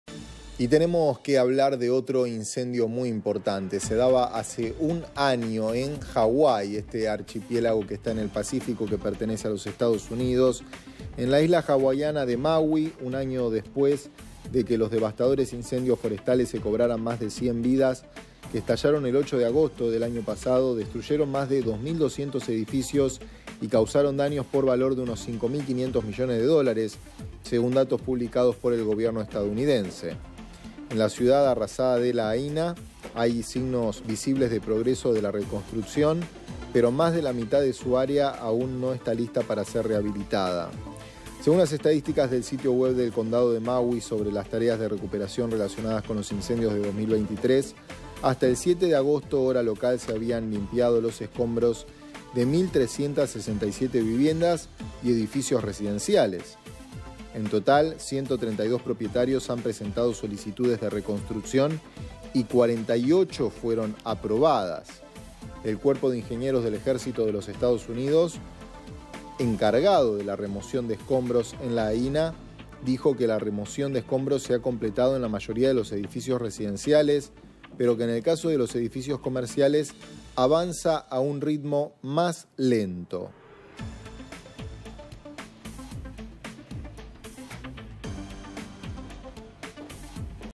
EEUU | A un año del incendio, continúa la remoción de escombros en Hawái
👉 En la isla hawaiana de Maui, un año después de que los devastadores incendios forestales se cobraran más de 100 vidas, que estallaron el 8 de agosto del año pasado, destruyeron más de 2.200 edificios y causaron daños por valor de unos 5.500 millones de dólares.